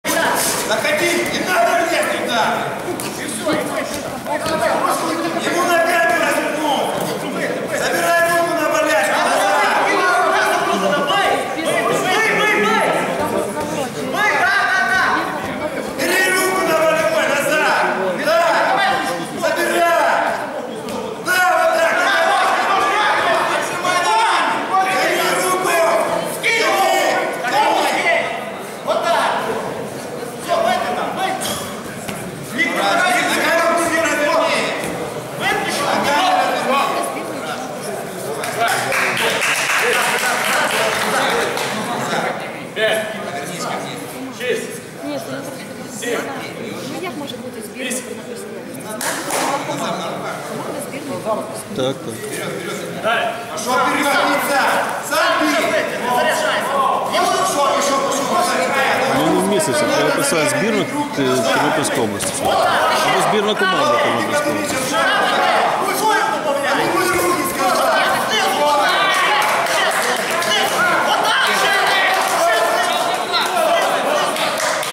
Петрусяк Назар - Онищенко Назар - 2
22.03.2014 року, м. Тернопіль, другий раунд поєдинку в рамках командної зустрічі 1/2 фіналу Кубку України з фрі-файту дивізіону Аматор між збірними командами Івано-Франківської та Полтавської областей у ваговій категорії -89 кг.

переміг Метрусяк Назар, ТКО, відмова секунданта від поєдинку.